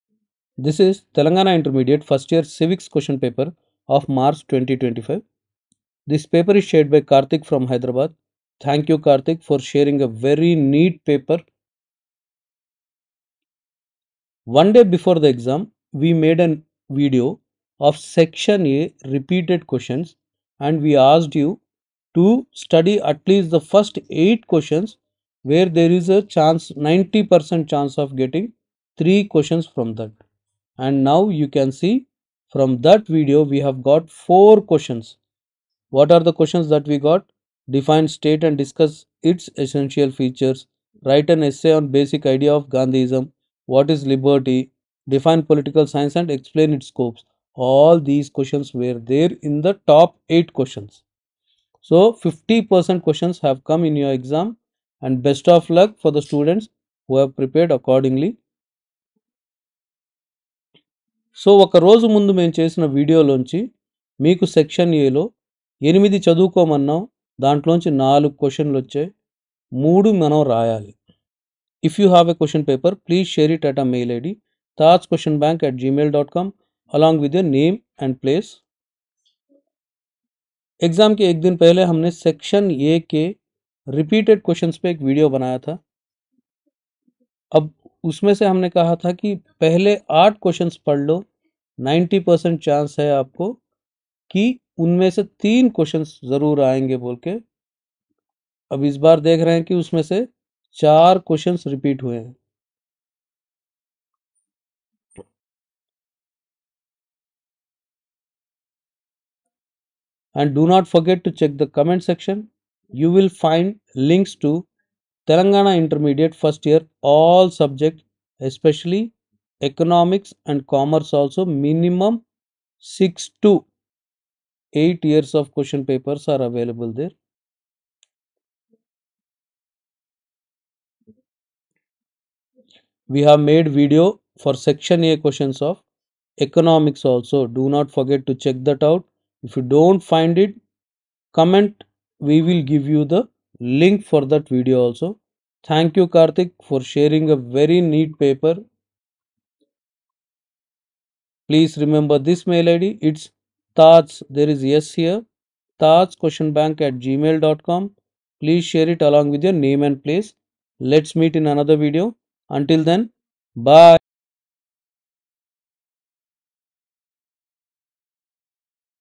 Telangana TS TG Inter 1st Year Civics 2025 March Previous year Question Paper Physical science
2025 March Civics question paper of Telangana intermediate 1st year Political Science. Telangana inter past papers or previous year papers.